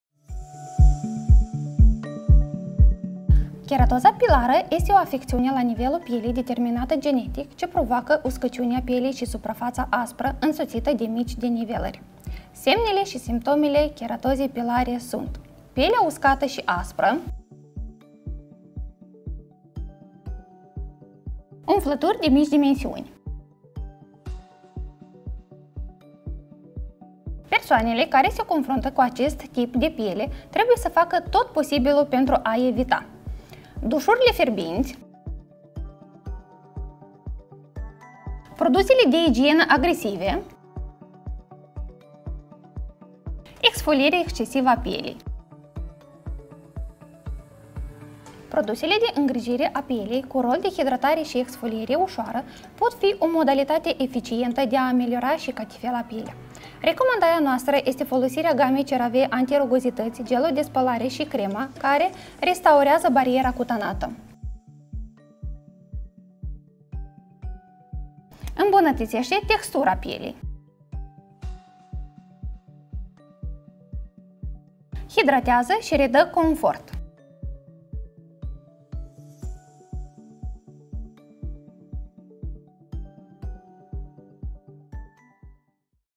DermatoAcademy - Keratoza pilară
Continuăm seria de videouri educaționale DermatoAcademy, cu experți în dermatologie. În episodul de astăzi, dr. Elena Ungureanu, medic dermatolog, ne vorbește despre Keratoza pilară - o afecțiune ce provoacă uscăciunea pielii. Descoperă semnele și simptomele acesteia, dar și metoda eficientă de ameliorare a keratozei pilare.

Îți dăm un pont - în rețeaua Farmacia Familiei găsești produsul conceput special pentru această afecțiune cutanată: crema antirugozități din gama CeraVe.